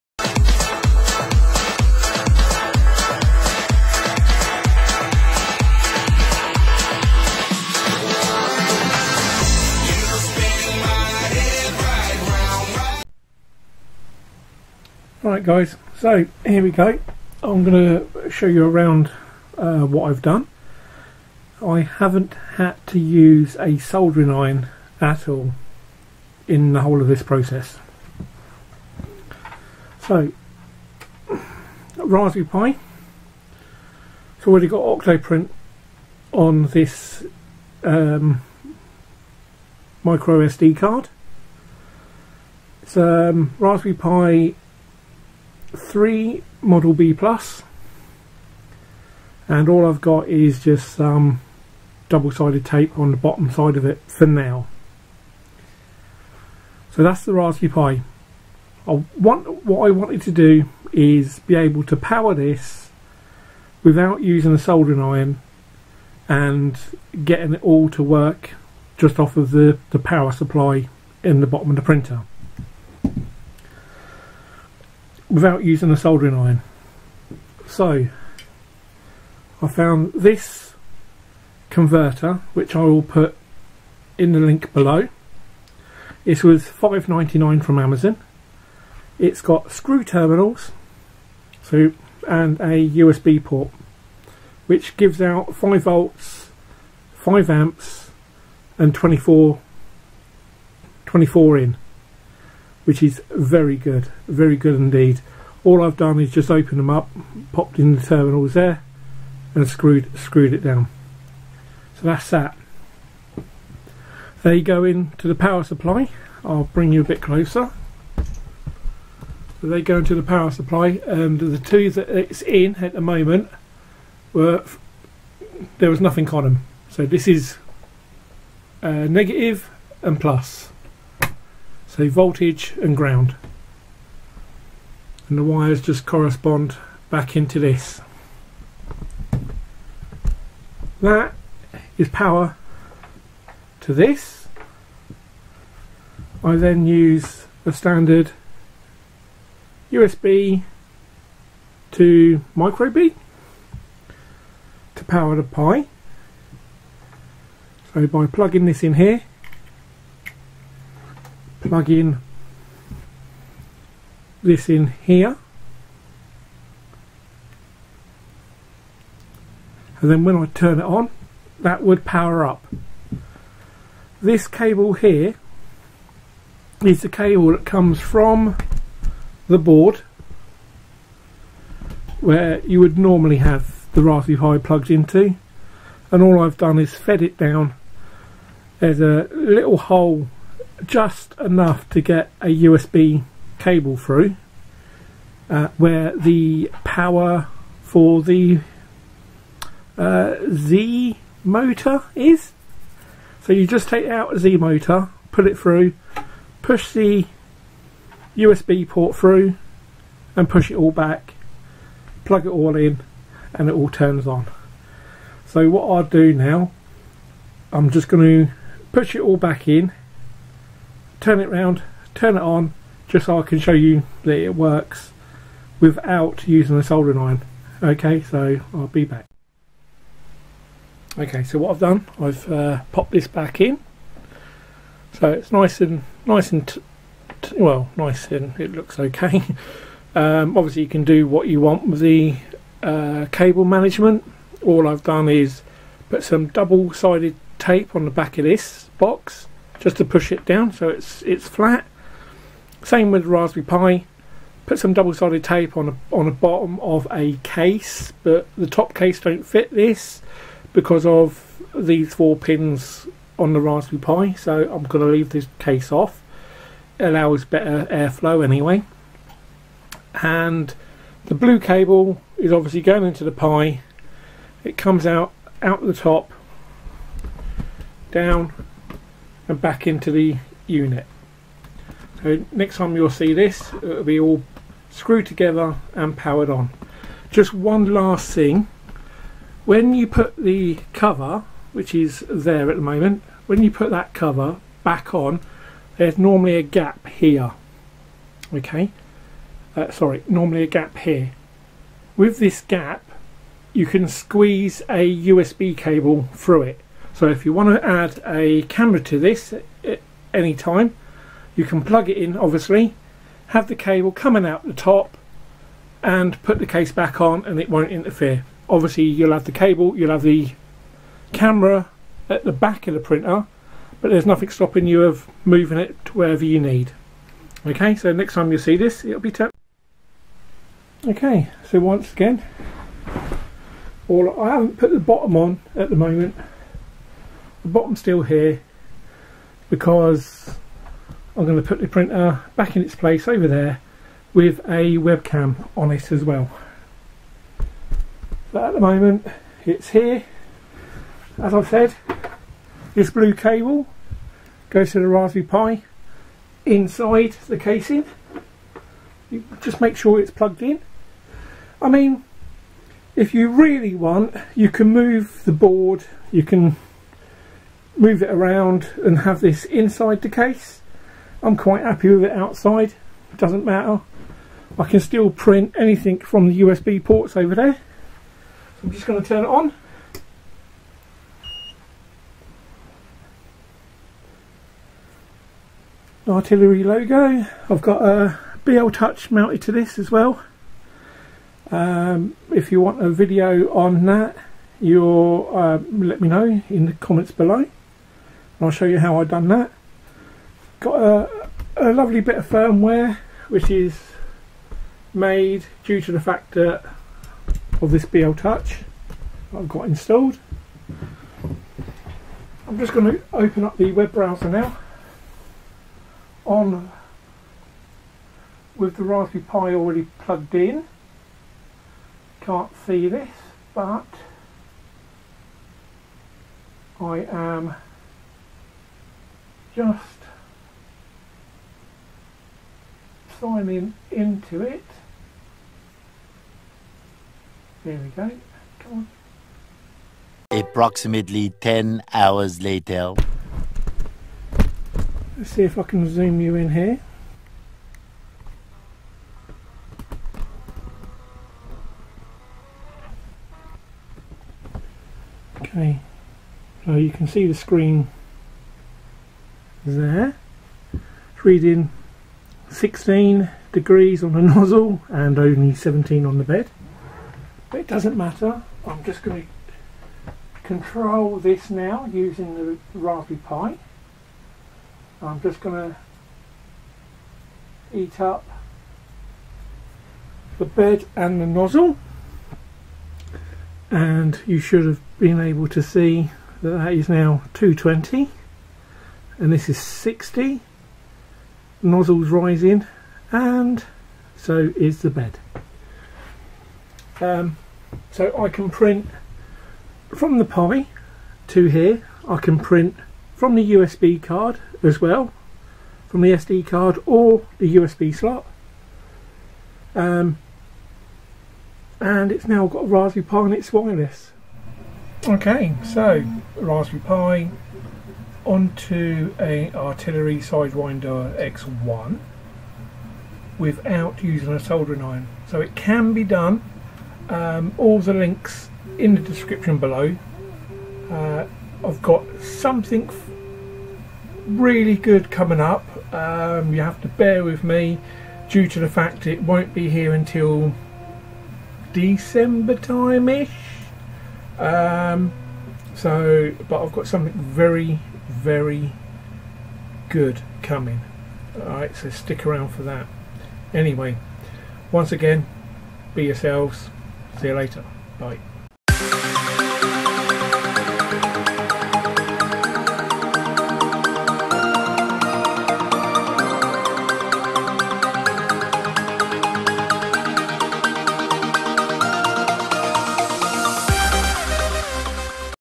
Artillery Sidewinder SX1 with Octoprint. No Soldering Iron in sight.
Hey guys,

So I was thinking, is it possible for someone, who doesn't own a soldering Iron to be able to install a Raspberry Pi for Octoprint using the power from the power supply.  This is what I came up with.

Parts needed:

24v - 5v 5A Buck converter

A Raspberry Pi case slim.

Shout out to EtherealProject3D for providing me this board.

tell them I sent you for a discount off your first order.